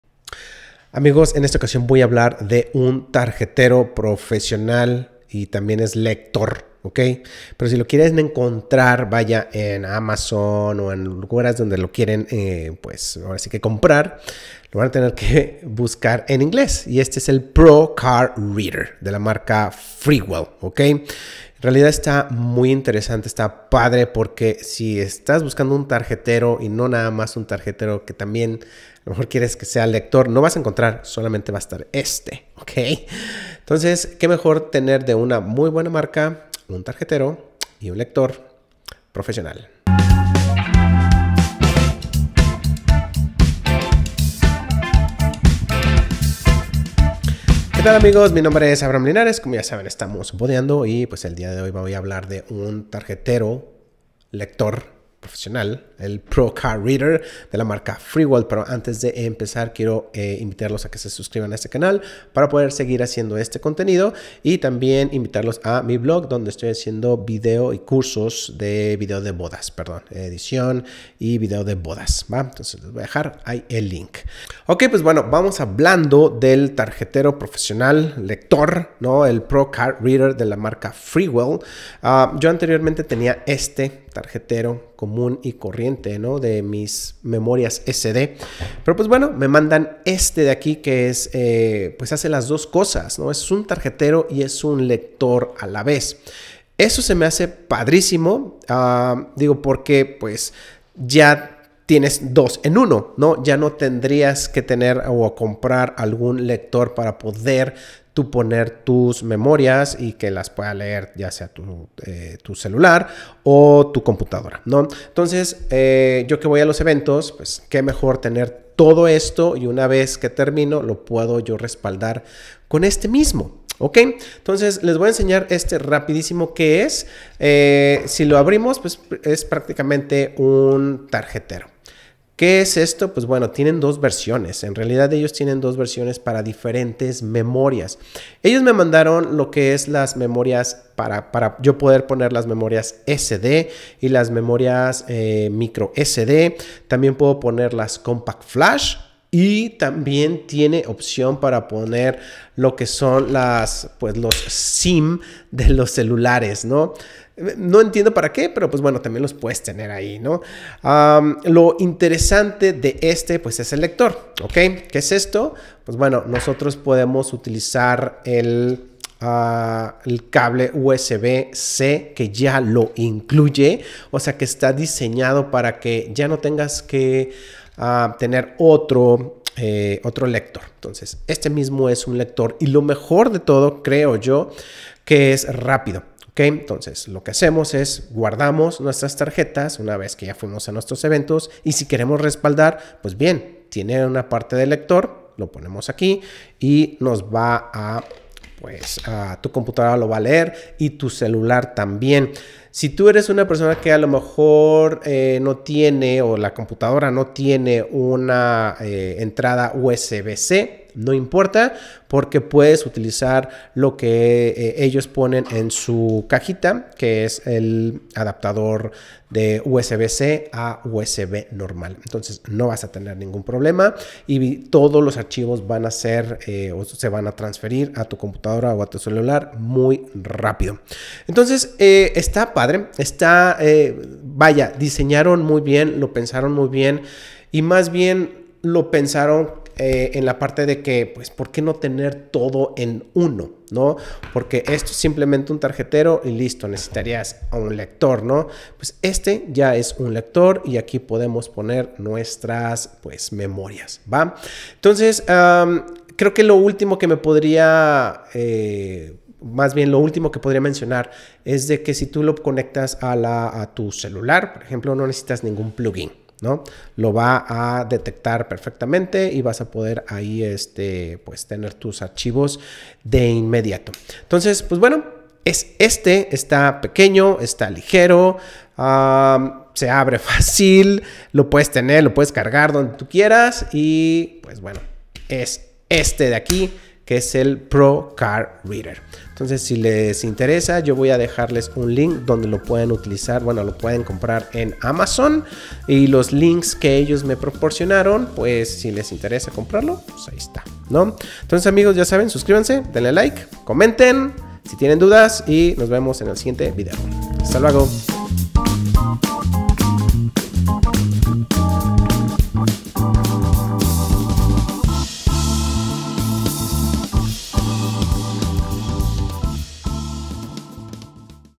El PRO CARD READER de FreeWell- Tarjetero y Lector Profesional
Que mejor tener dos en uno y que sea ultra rápido, con el PRO READER de Freewell

Link para comprarlo en Amazon Mod.FW-CR01

Link para comprarlo en Amazon Mod. FW-CR02-PRO

Link para el Website
SKU: FW-CR01

También por Aliexpress

SKU: FW-CR01
SKU: FW-CR02-PRO

Aquí les dejo mis trabajos